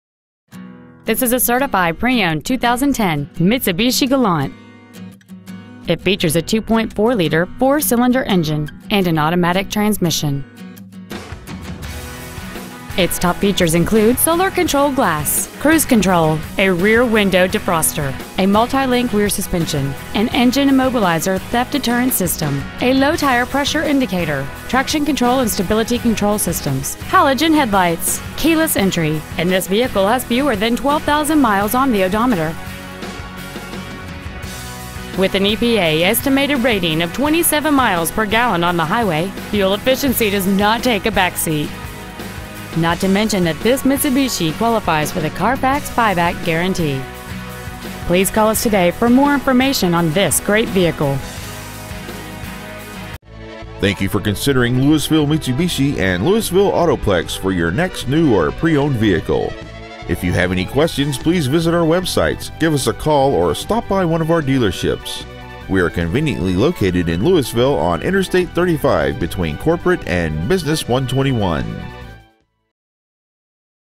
2010 Mitsubishi Galant Certified Carrollton TX
Year : 2010
 Make : Mitsubishi
 Model : Galant
 Engine : 2.4L SOHC MPFI MIVEC 16-valve I4 engine
 Trans . : Automatic
 Exterior : White
 Miles : 11,566
 Interior : Gray
 Stock : 7753

 2010 Mitsubishi Galant Lewisville TX